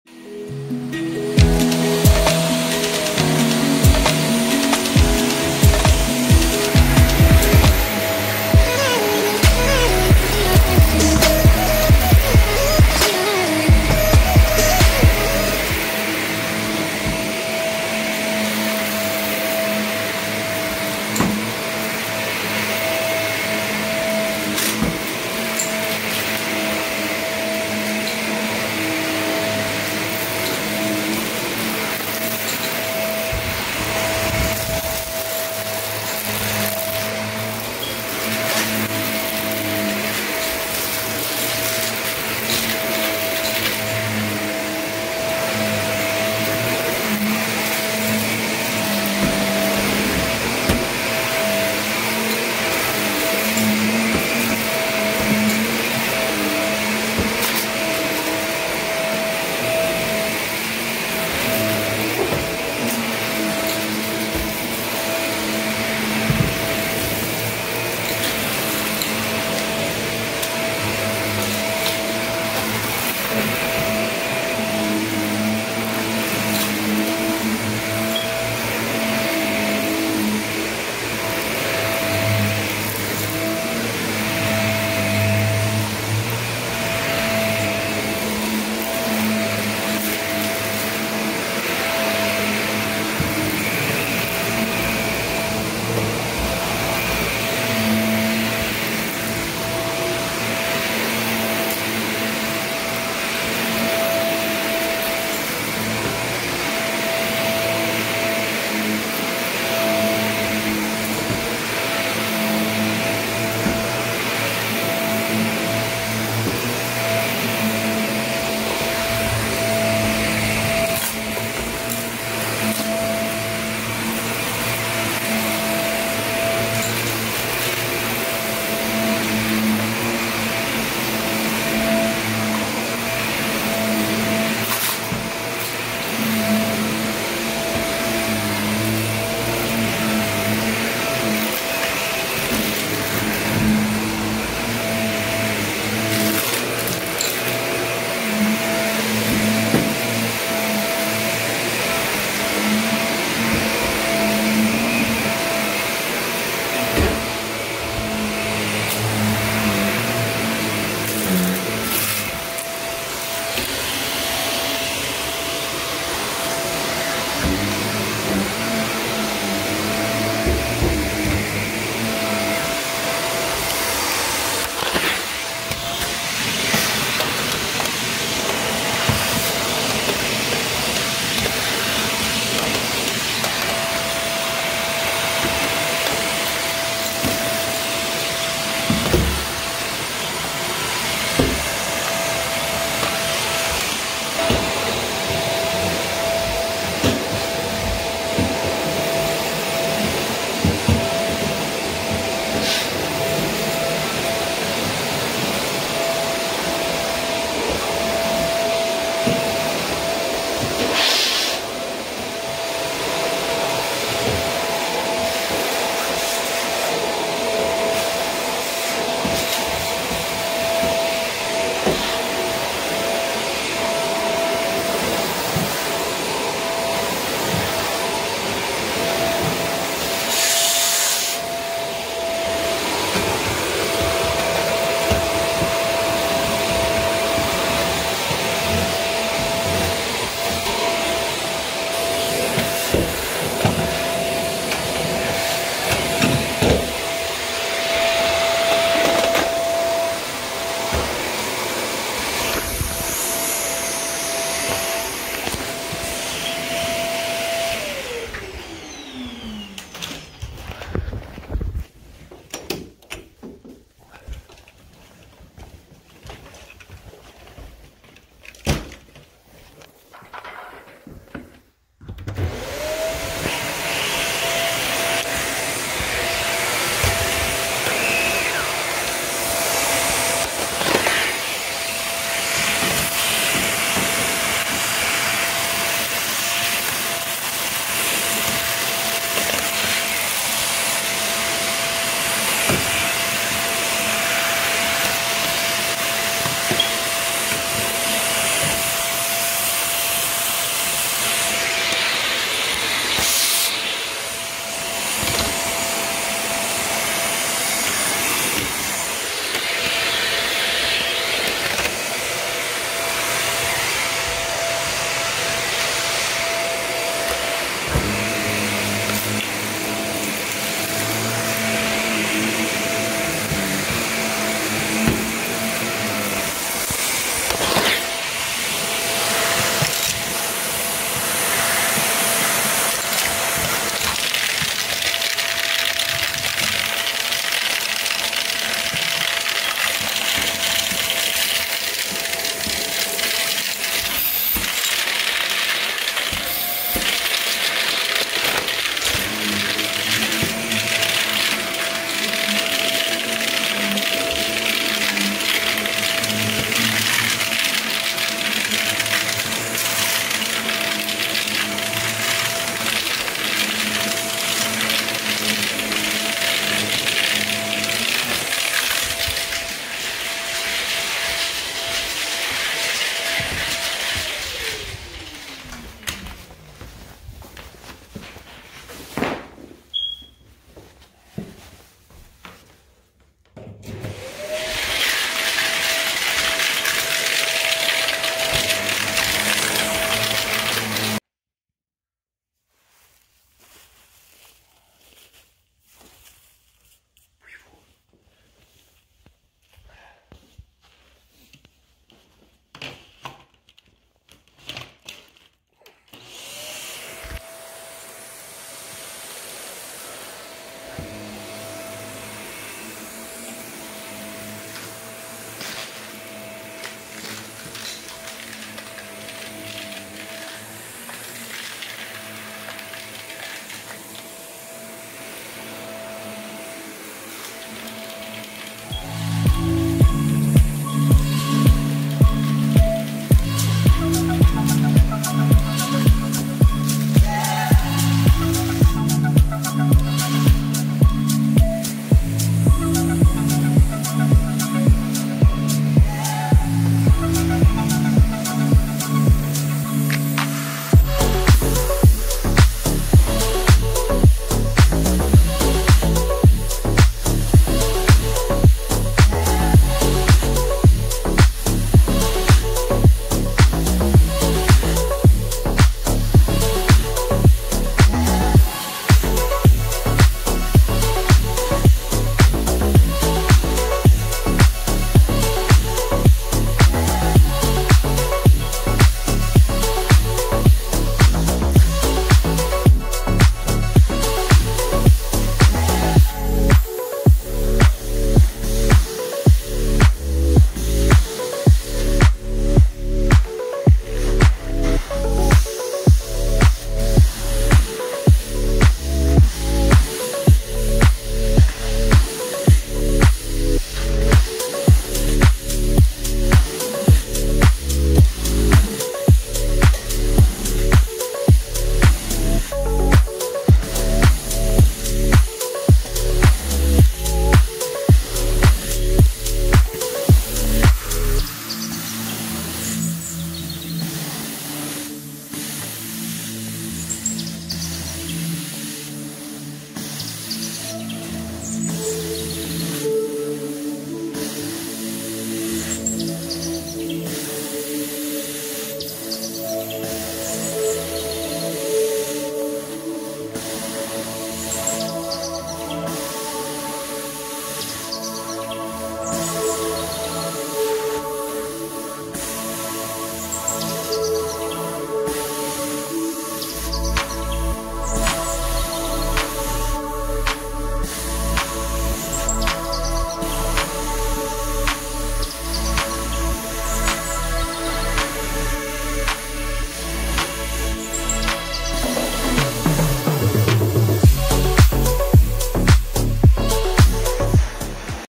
Vacuuming vlog #4 Heavy Soil!
New client in ozone park, Queens New york, unsure how she found us.  Duplex home, cleaning the apartment carpet getting the unit ready for rent.  Huge stains, plenty of debris, heavily soiled traffic areas!